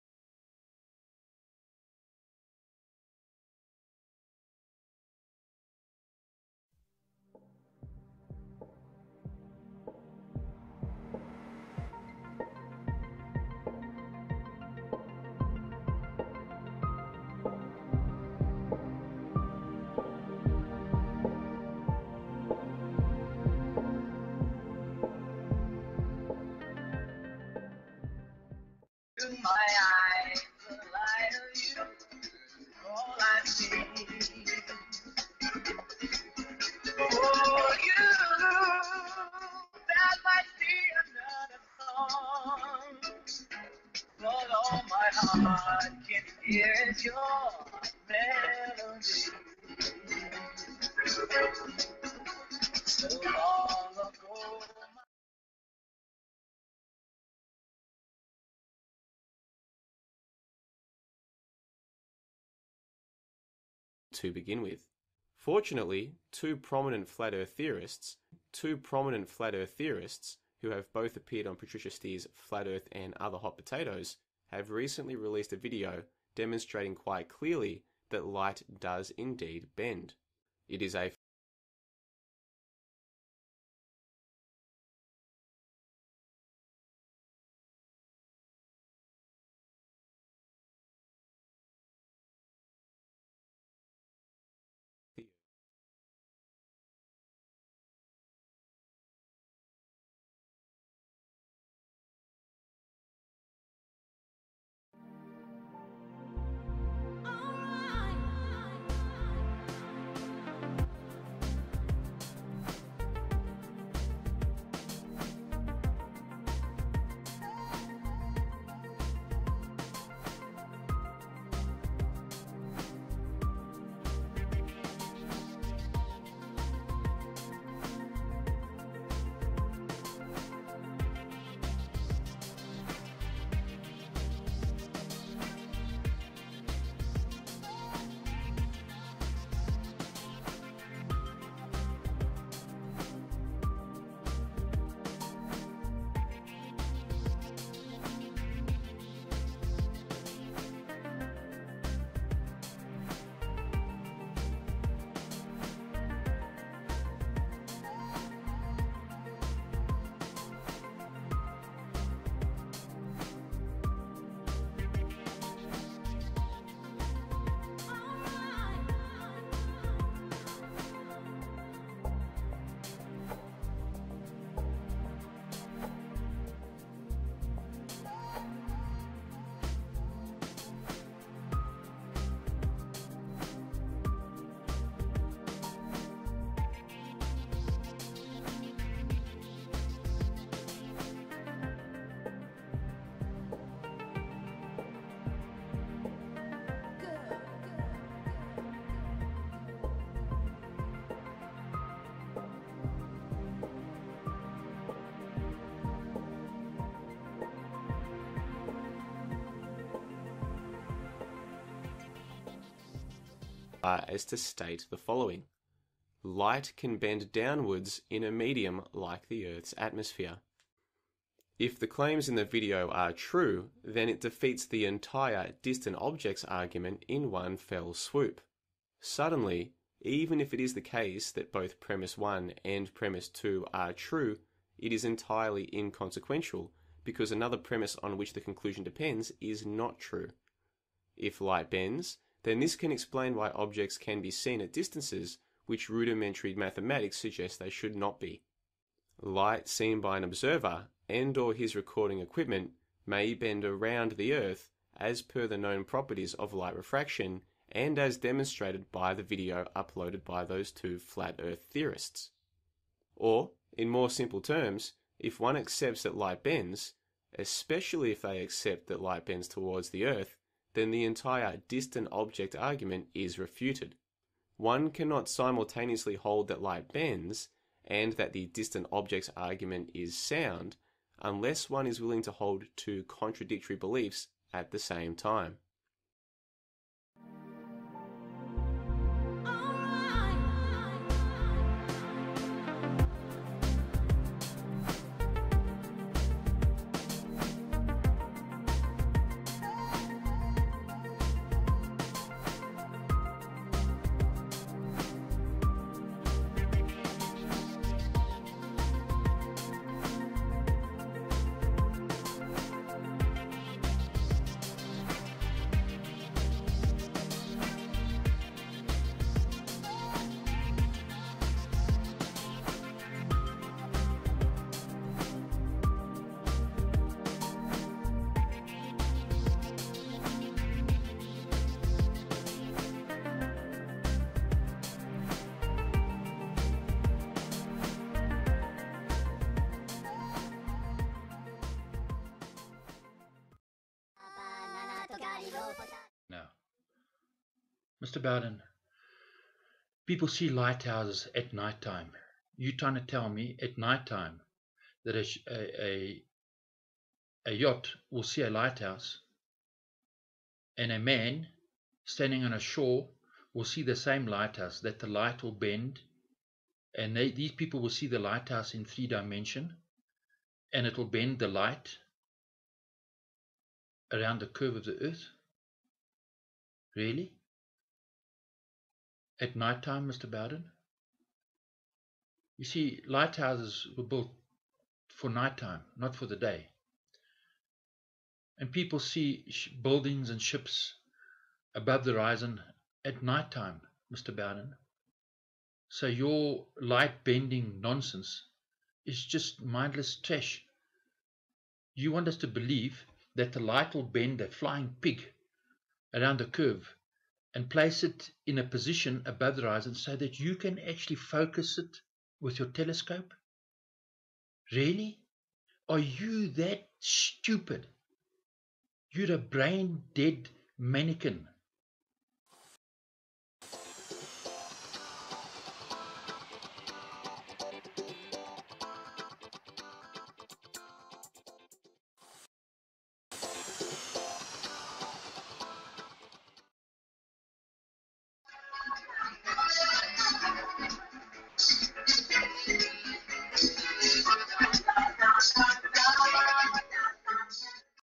JLB and Matrix Decode gatekeeping hard - Light bending in the air - part 1
Clips:
by My Perspective

FIG.4.1 Mirage froid en Antarctique
(Crédit: Y.Sallaz-Damaz)

Departament of Atmospheric and Oceanic Sciences, University of Wisconsin - Madison

Copyright Ctein, glidder image

Chicago Skyline by Joshua Nowicki

Join Matrix Decode, David Weiss and host John le Bon